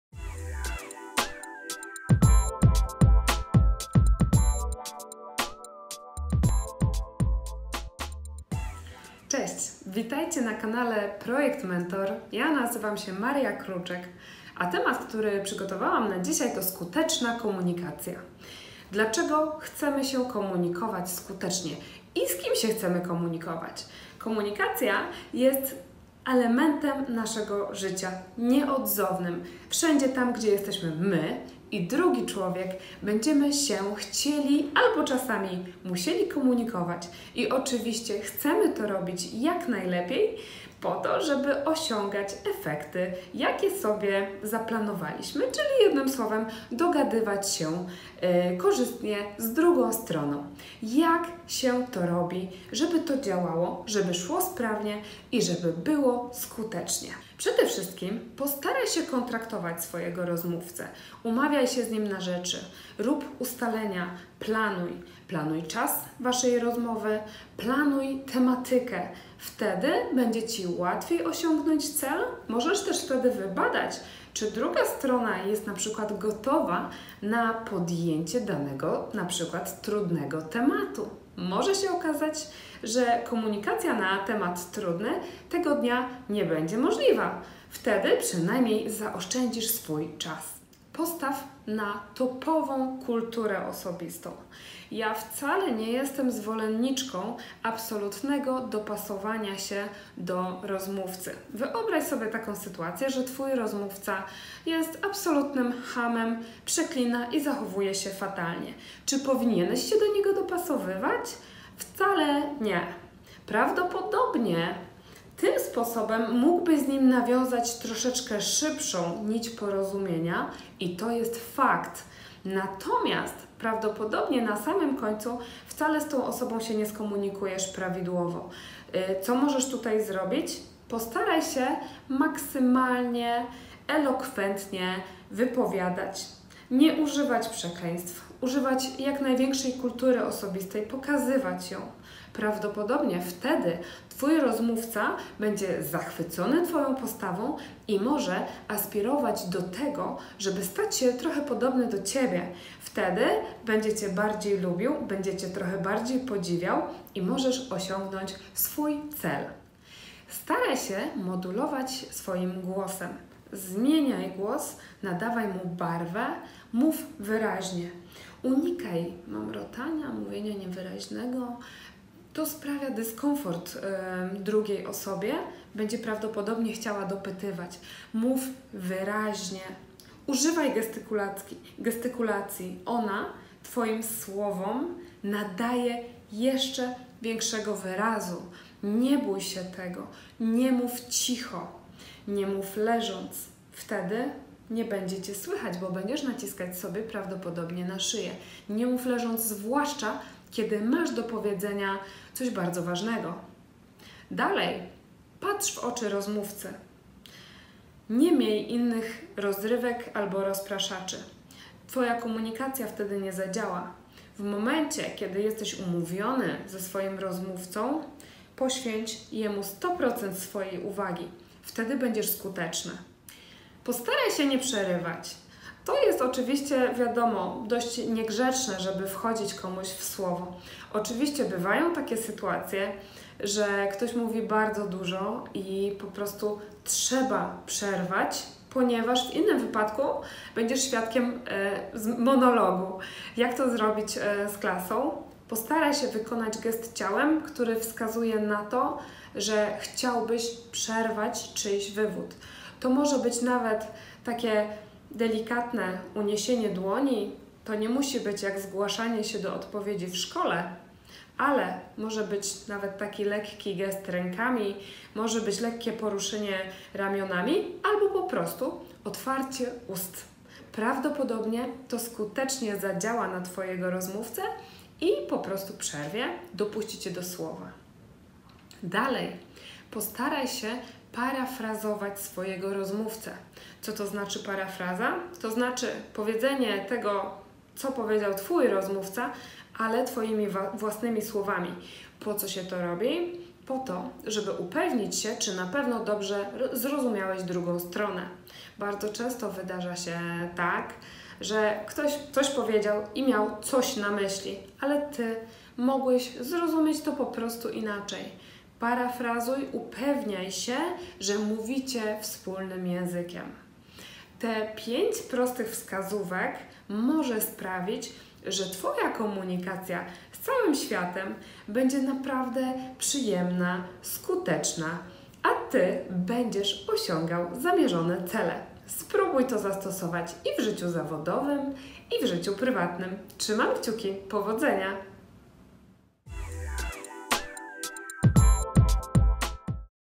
Skuteczna komunikacja nie tylko w biznesie. HR, consulting, Projekt Mentor
Tyle się mówi o komunikacji, a fakt jest taki, że znam naprawdę  niewiele osób, które potrafią komunikować się z empatią, spokojem i do tego SKUTECZNIE. Może to dlatego, że kiedy coś jest dla nas ważne, często ponoszą nas emocje? Warto je trochę poskromić, zastosować kilka rad z tego filmu i zobaczyć, czy nagle nie okaże się, że twoje życie jest piękniejsze, konfliktów mniej a załatwionych spraw- więcej?
Kolejna kwestia jest taka, że jeśli narzekasz na słabe efekty komunikacji (np "nie dogaduję się z parnterem") to pamiętaj, że jedyny sposób, żeby coś na to poradzić to wprowadzenie zmian. Zadziała to tylko wtedy, gdy skupisz się na sobie i zmiany zaczniesz od siebie:)

tym bardziej polecam to video !